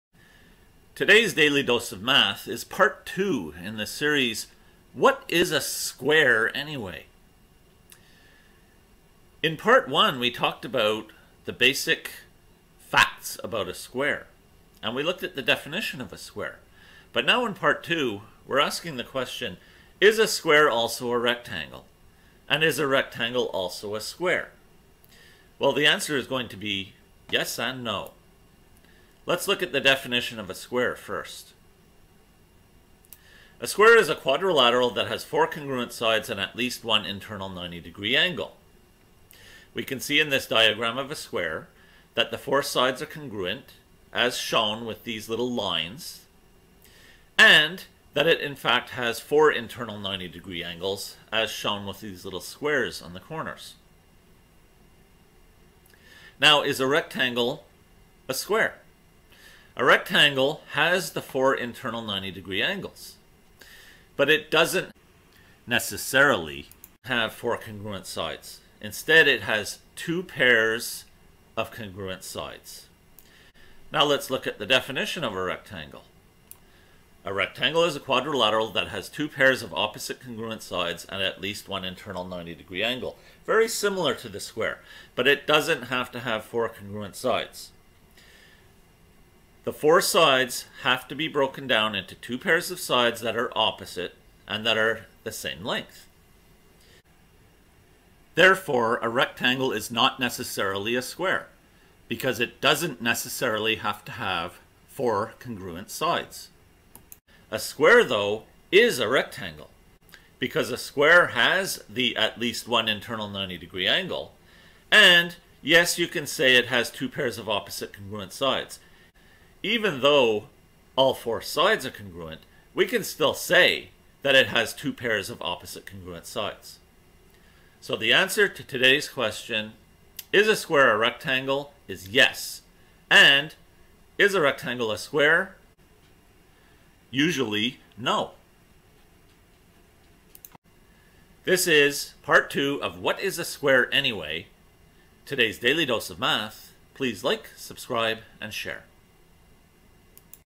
What is a SQUARE, anyway? Part 2: Is a Square a Rectangle? Is a Rectangle a Square?
The second in a series of videos about the wonderful shape we call the square.